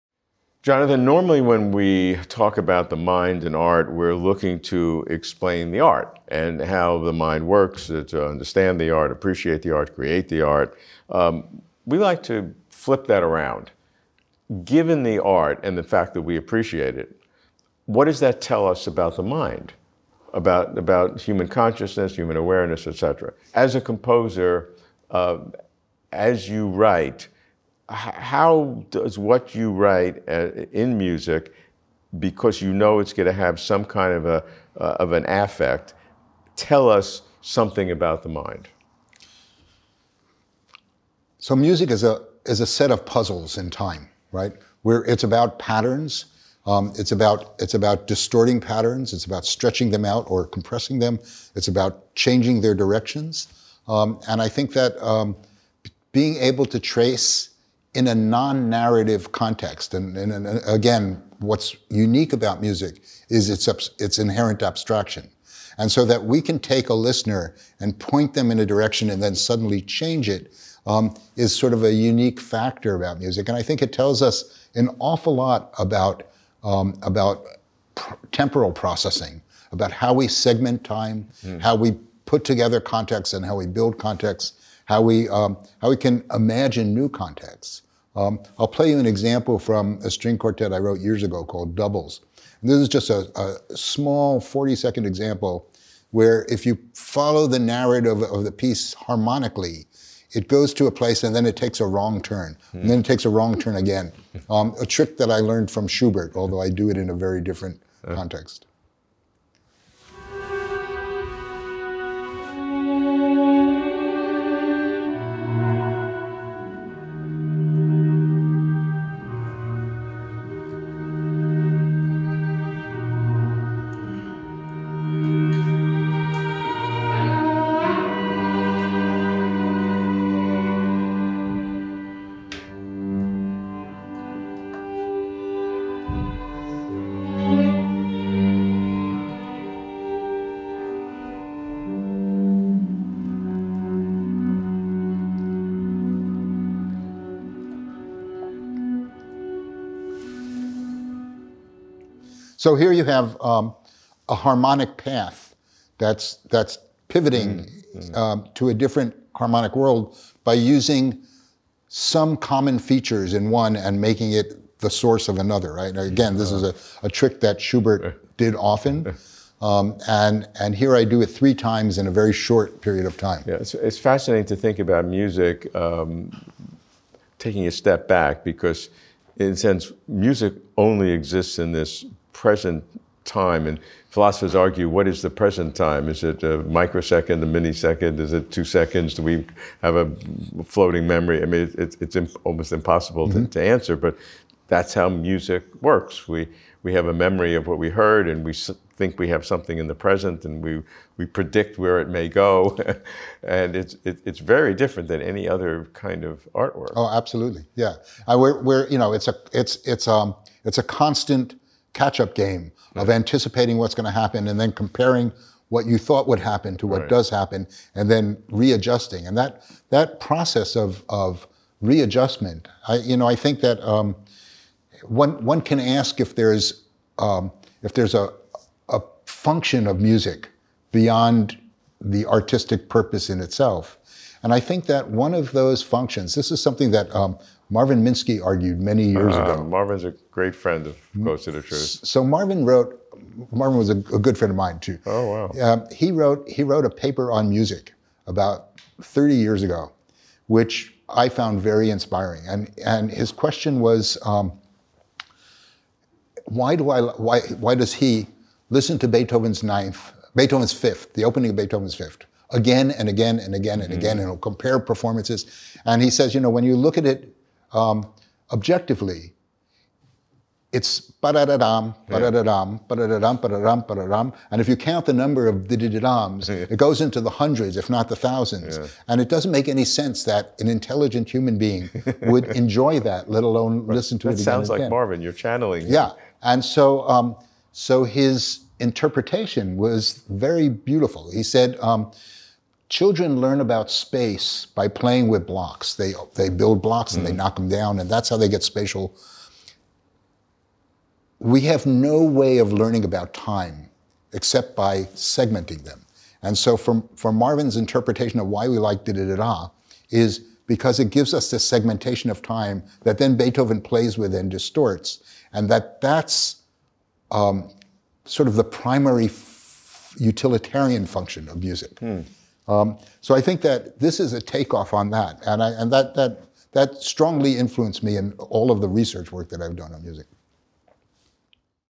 Jonathan Berger - Using Art to Understand the Mind
What can art, including music, reveal about how the mind works? How does cognitive science study the human capacity for, and experience of, music? How to tease apart the elements of music: rhythm, melody, harmony, dynamics, tone color, etc., and how are all the elements integrated into a coherent, flowing whole? How can art help us understand the mind?

Jonathan Berger is a composer. His works include opera, orchestral, chamber, vocal, choral and electro-acoustic music.